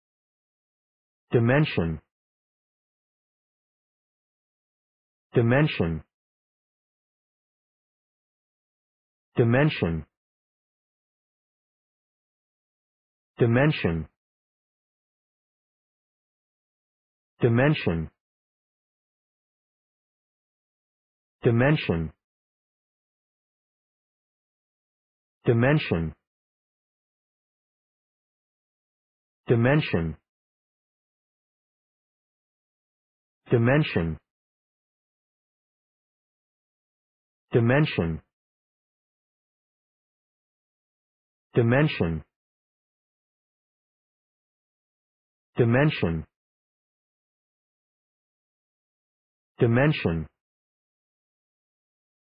Pronunciation of Dimension | Definition of Dimension
Dimension pronunciation | How to pronounce Dimension in English?
/dɪ`menʃən/
Meaning of Dimension | What is Dimension?
(1) (noun) a construct whereby objects or individuals can be distinguished; (2) (noun) magnitude or extent; (3) (verb) indicate the dimensions on; (4) (noun) one of three Cartesian coordinates that determine a position in space; (5) (verb) shape or form to required dimensions; (6) (noun) the magnitude of something in a particular direction (especially length or width or height);
What is Dimension | Dimension meaning and definition?